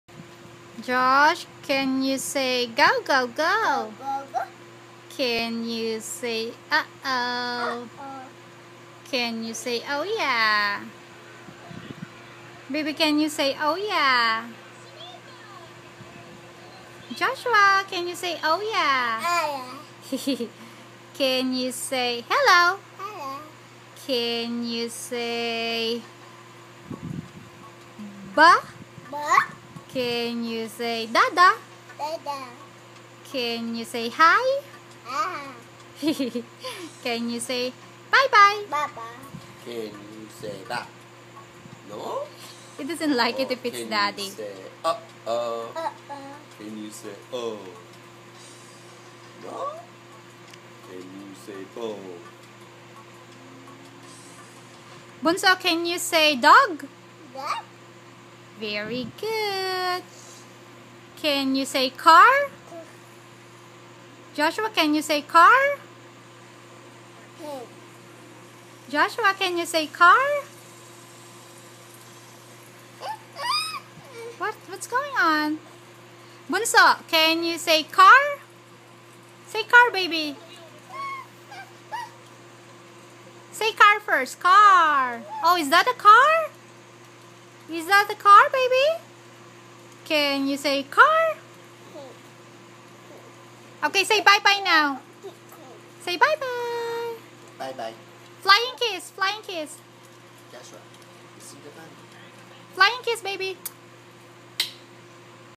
Joshua Practicing Saying Words While Busy Playing iPad - Toddler Practice Talking
My son sounds so cute. He doesn't really like it every time we practice him talking. He feels shy. Though he knows more or less 15 words already. Like Dada, Mama, Dog, Hi, Hello, Bye bye, Hot, Oh Yeah, Ball, Wow, Go go go, La la la, A awww).

iPhone 4S Video. The video looks dark coz we we're at the Patio out back. Almost bedtime but my boy still busy playing iPad.

Stop the hate. BE KIND. Support and Inspire each other 🤍❤️